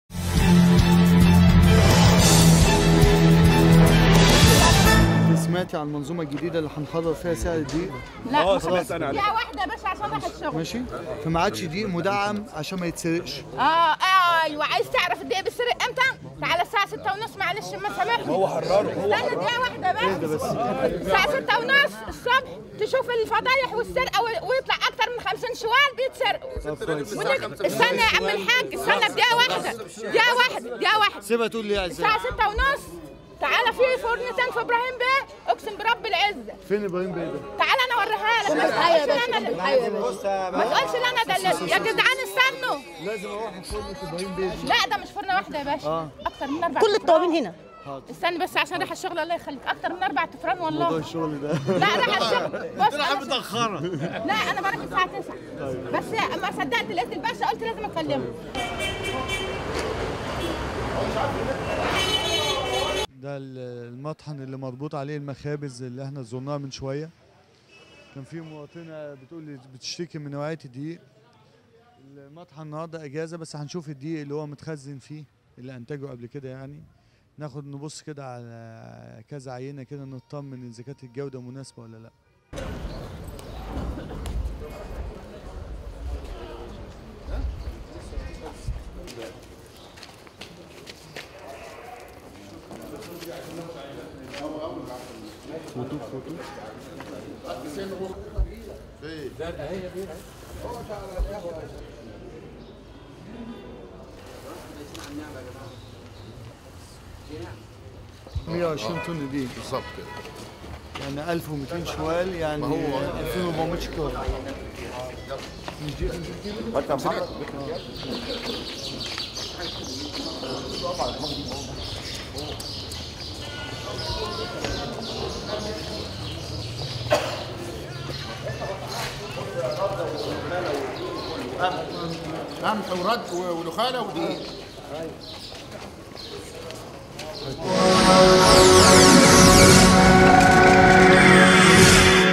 مواطنة تخبر الوزير بفساد الدقيق فيتوجه للتفتيش على المطاحن
رصد | د. باسم عودة - وزير التموين يستمع لشكوى مواطنة تخبره بفساد الدقيق فيتوجه مباشرة للتفتيش على المطاحن

تقرير : حسام زيدان
تصوير : عمر خضر
مونتاج : محمود محمد